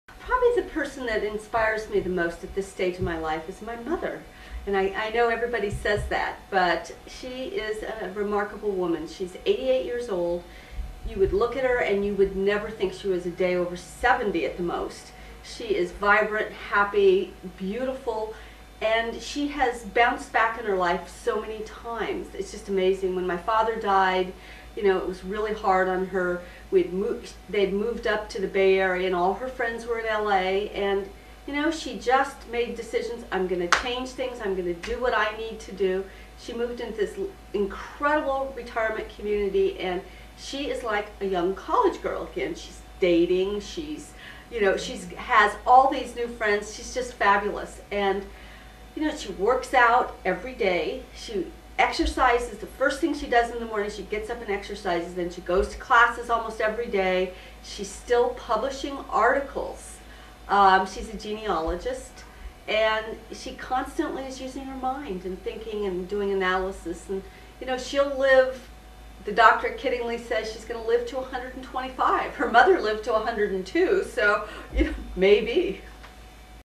Inspiration: Bay Area: Pam
Pam is inspired by her mom.